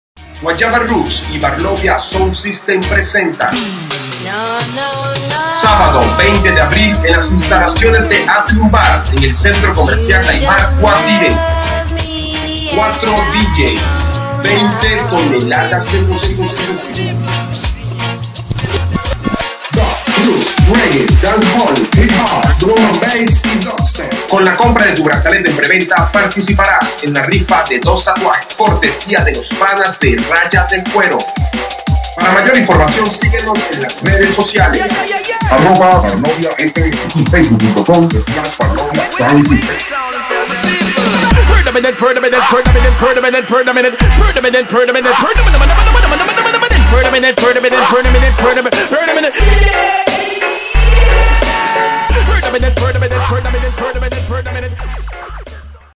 CUATROVEINTE -Promo-
GUAYABA ROOTS y BARLOVIA SOUND SYSTEM
...presentan:

CUATROVEINTE

Sabado 20 de Abril de 2013
En las instalaciones de

ATRIVM BAR
C.c Daymar - Guatire
(Frente a la Churuata del Conejo)